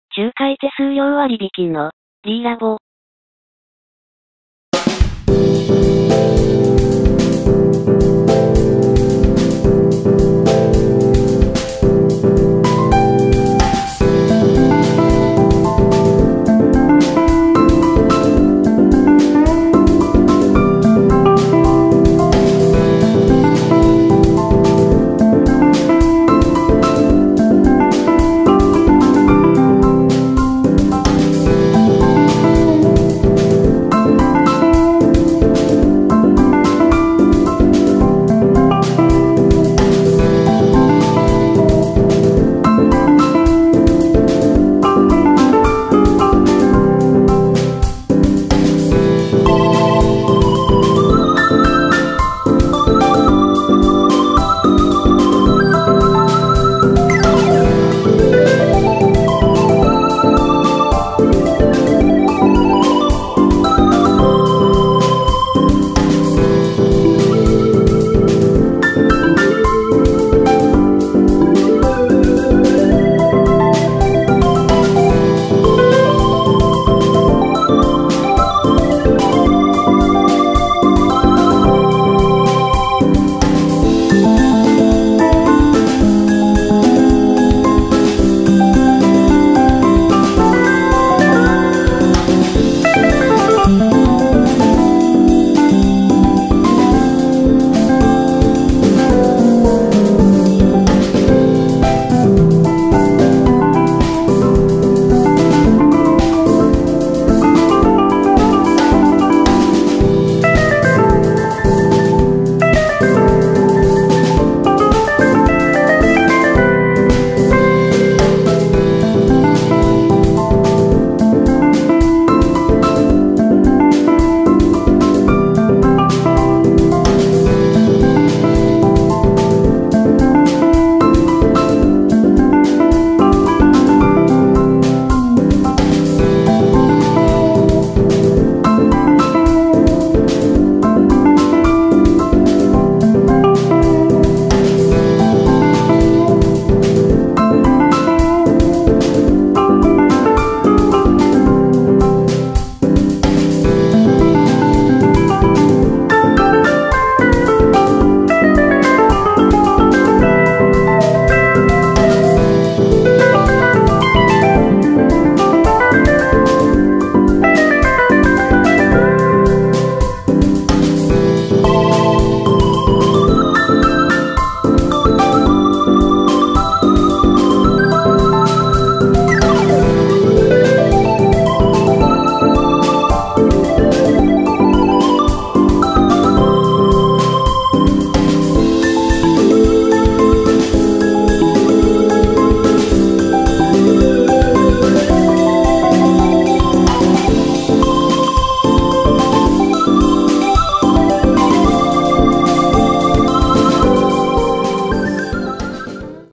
宮崎市 デザイナーズ賃貸部屋 おしゃれな一人暮らし向けマンション VALO吉村 1号【不動産のリーラボ】
宮崎市吉村町ワンルーム賃貸物件。VALO吉村 空室情報